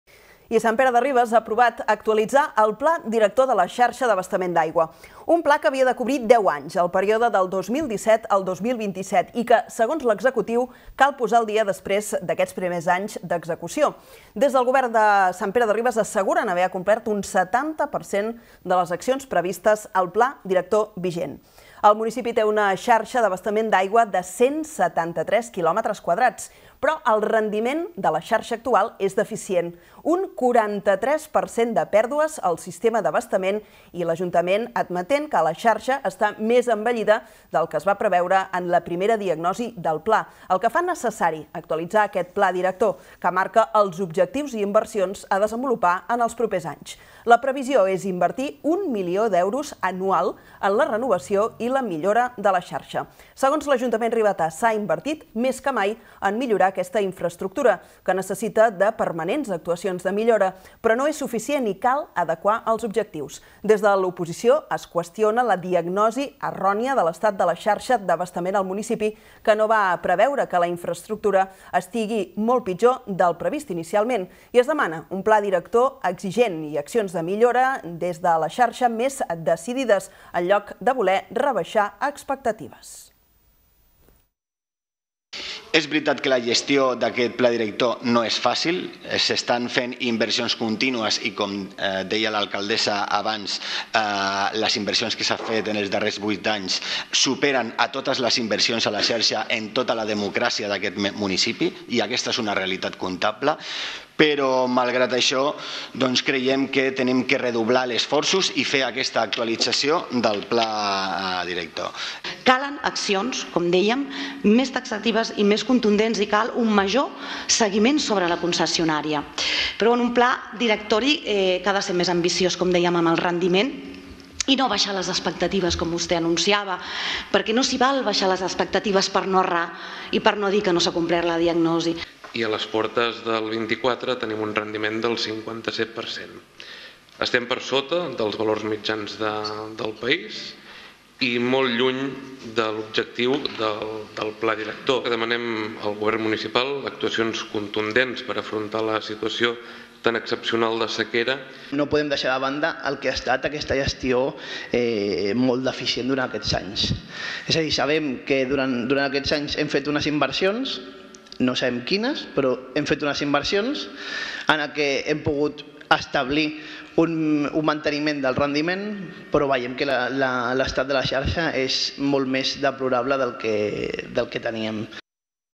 Sant Pere de Ribes actualitza el Pla Director de l'Aigua
Sant Pere de Ribes actualitzarà el Pla Director de la xarxa d’abastament d’aigua. Un pla que havia de cobrir  deu anys -el període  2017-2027-  i que segons l’executiu, cal posar al dia després d’aquests primers anys d’execució. Des del govern ribetà asseguren que han completat un 70% de les accions previstes al Pla director vigent, en un municipi amb una xarxa d’abastament d’aigua de 173 kilòmetres quadrats.

Però el rendiment de la xarxa actual és deficient: un 43% de pèrdues al sistema d’abastament  i l’ajuntament admet que la xarxa està més envellida  del que es va preveure en la diagnosi del Pla, el que fa necessari actualitzar el Pla director, que marca els objectius i inversions a desenvolupar en els propers anys. La previsió del govern és d'1 milió d'euros anual en inversions.

Segons l’Ajuntament de Sant Pere de Ribes, s’ha invertit més que mai en millorar aquesta infraestructura, que necessita de permanents actuacions de millora, però no és suficient i cal adequar els objectius.

Des de l’oposició es qüestiona la diagnosi errònia de l’estat de la xarxa d’abastament al municipi i que no va preveure que la infraestructura estigués molt pitjor del previst inicialment. També es demana un pla director exigent enlloc de voler rebaixar expectatives.